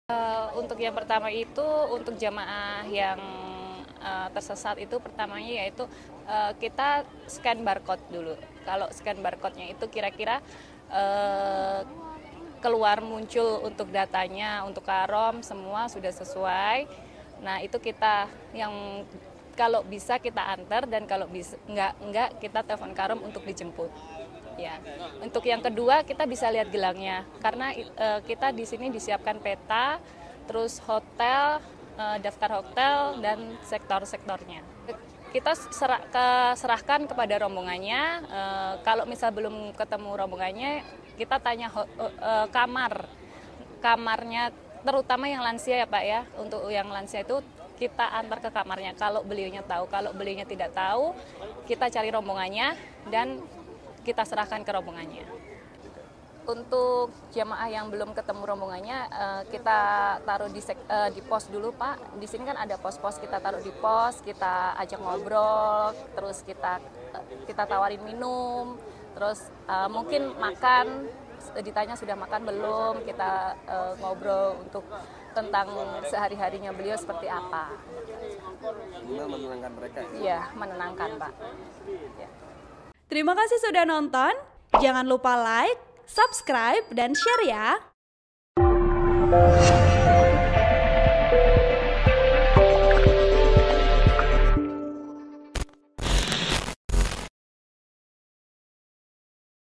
Pos-pos di Masjid Nabawi Sukses & Lancar Beri Layanan, Bantu Para Jemaah Haji yang Tersesat
TRIBUN-VIDEO.COM - Pos-pos di sektor khusus (seksus) Masjid Nabawi menjadi tempat tujuan utama jemaah haji Indonesia untuk mencari bantuan agar bisa kembali ke rombongan.

Ada lima pos di sektor khusus Masjid. Di setiap sektor, ada petugas dari sejumlah layanan yang ada di dalam Petugas Penyelenggara Ibadah Haji (PPIH) Arab Saudi.

Mulai dari petugas perlindungan jemaah (linjam), Penanganan Krisis dan Pertolongan Pertama Jemaah (PKP3JH), layanan lansia dan sebagainya.

Sektor khusus menjadi penolong jemaah haji Indonesia yang tersesat. Apalagi, lansia. Mereka akan dilayani dan diantar sampai kembali ke pemondokannya atau hotel.

Nuari Azizah, salah satu petugas dari Sekus Masjid Nabawi mengatakan, jika ada jemaah yang tersesat, petugas akan berusaha menenangkan jemaah itu terlebih dahulu.

“Biasanya orang kalau merasa tersesat dan bingung jalan pulang, mereka akan panik. Nah itu , ketika ada jemaah, kami persilahkan duduk dulu,” katanya, Rabu (7/6/2023).

Setelah itu, ia mengecek kartu jemaah yang ada scan barcodenya. Disitu , tercantum informasi lengkap nama jemaah, lokasi hotelnya dan ketua kloternya.

“Tapi biasanya banyak yang tidak membawa kartu jemaah ini. Kebanyankan itu tertinggal di kamar hotelnya,” sambung Azizah, sapaan akrabnya.

Jika kartu jemaah tertinggal, lanjut dia, petugas seksus akan mengecek gelang jemaah. Di gelang juga tercantum kloter, embarkasi dan nama jemaah.

“Kami dibekali buku khusus yang mencantumkan embarkasi, hotelnya dan semua informasi jemaah haji. Itu memudahkan kami,” paparnya.

Selanjutnya, kata dia, jika sudah ketemu hotelnya, dan rombongannya, petugas seksus akan menghibur para jemaah lansia ini.

“Ya minimal sembari menunggu jemputan, jemaah tidak bosan dan jenuh. Kami ajak ngobrol, diberi minuman atau makanan,” tegasnya.

Sesekali, kata dia, tak jarang juga petugas seksus harus mengantarkan jemaah haji lansia yang belum dijemput oleh ketua rombongannya.

“Ya kami antar sampai ke hotelnya. Bisa jalan kaki, atau didorong pakai kursi roda. Kami sesuaikan dengan kondisi jemaah,” paparnya

Intinya, kata dia, seksus Nabawi akan mengantarkan jemaah kembali sampai ke kamar hotelnya. “Kami akan melindungi dan melayan jemaah haji yang tersesat,” tutupnya. (lih)